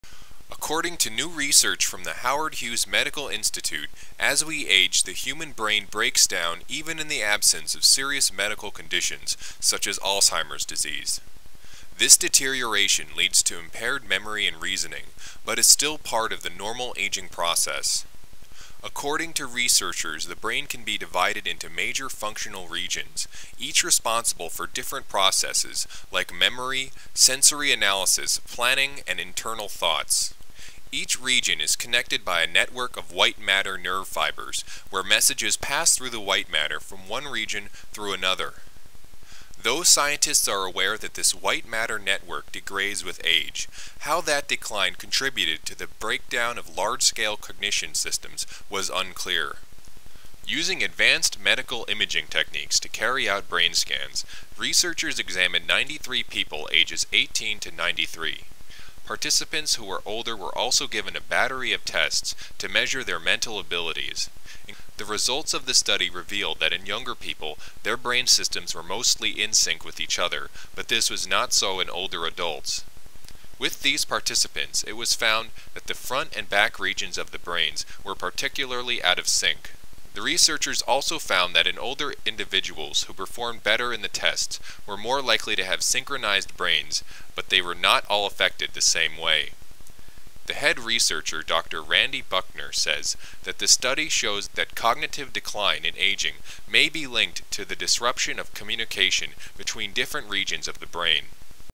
Empowered Health News | How Our Brain Changes With Age
According to new research from the Howard Hughes Medical Institute, as we age the human brain breaks down even in the absence of serious medical conditions, such as Alzheimer's disease. This deterioration leads to impaired memory and reasoning, but is still part of the normal aging process. According to researchers, the brain can be divided into major functional regions, each responsible for different processes like memory, sensory analysis, planning, and internal thoughts. Each region is connected by a network of "white matter" nerve fibers, where messages pass through the white matter from one region through another. Though scientists are aware that this white matter network degrades with age, how that decline contributed to the break down of large-scale cognition systems was unclear.

Story is produced and provided by Empowered Medical Media, LLC
to see the full story